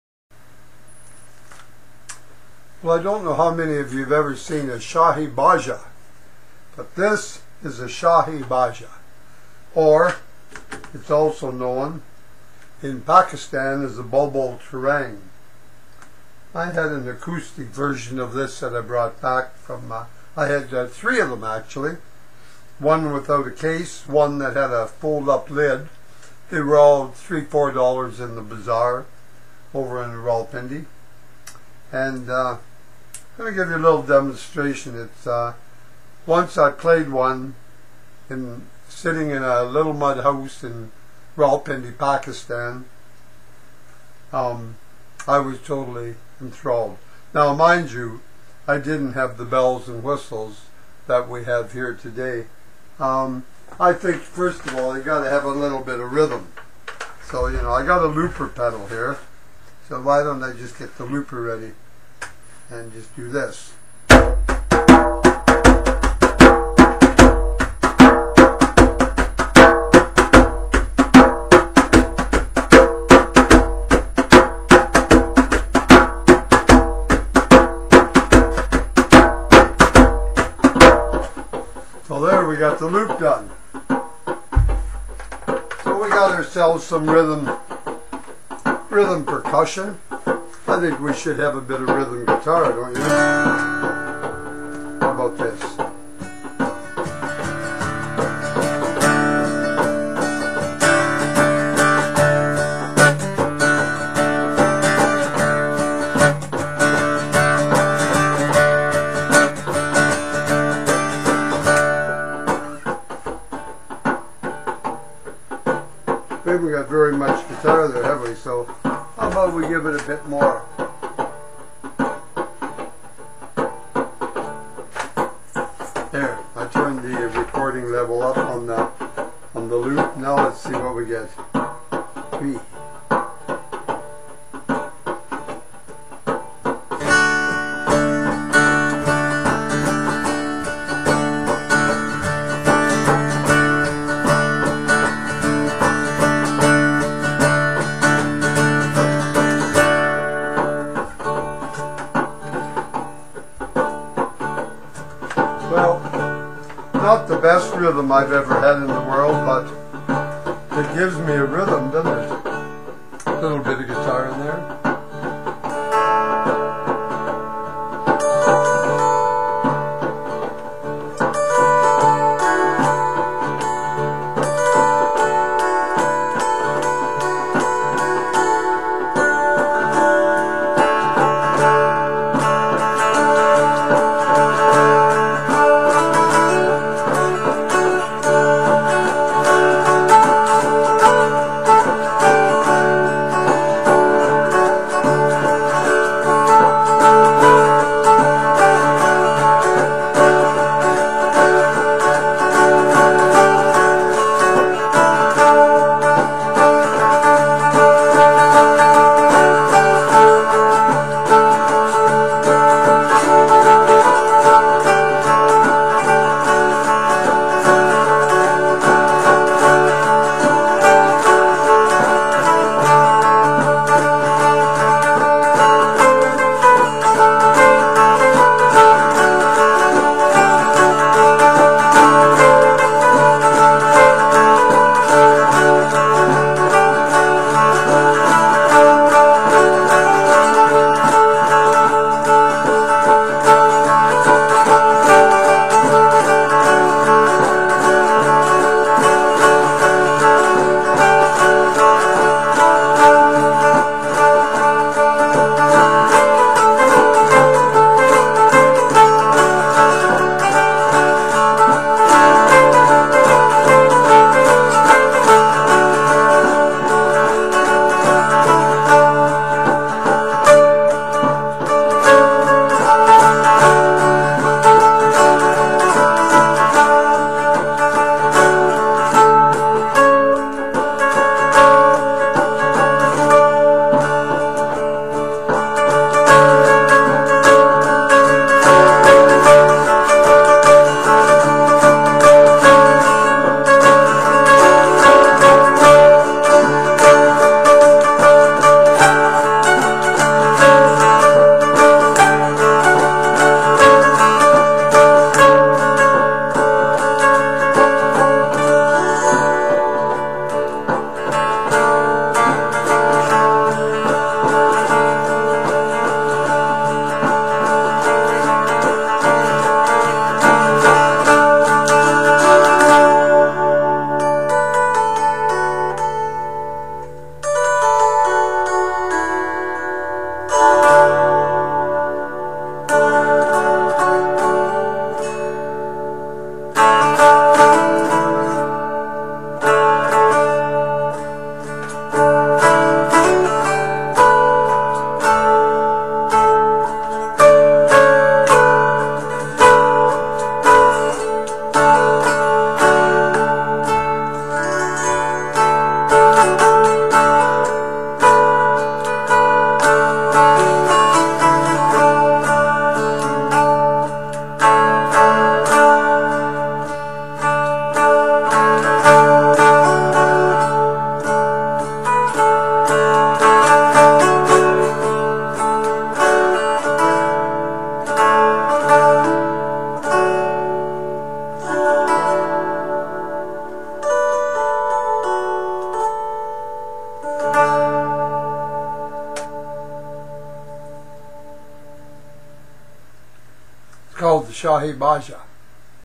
shaahi baja a k a bulbul tarang demo
I have a couple old acoustic instruments we brought home from Rawalpindi in 1969. One has typewriter keys for the 'keys' . The other was built into a box with a lid that closed for carrying. Not sure if the name 'bulbul tarang' is Urdu or Punjabi. 'Shaahi Baja' is an Indian word .When I saw this electric instrument on the net I had to have one. ;-)